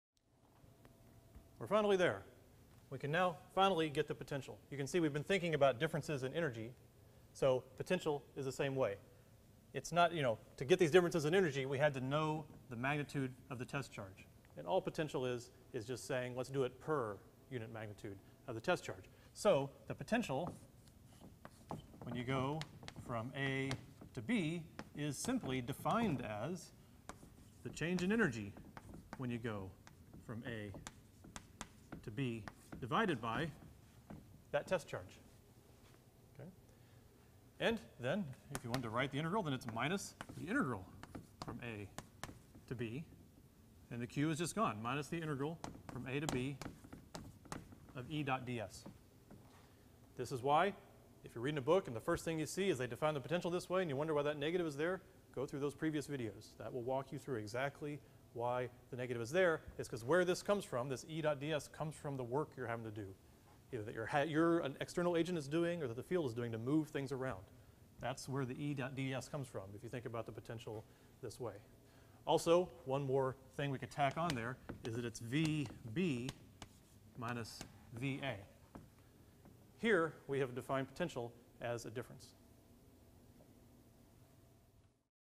PHYS 102 | Point Charge 4 - From Potential Energy to just plain Potential
Up to now, we have only covered electric potential energy.  Here we get to the electric potential.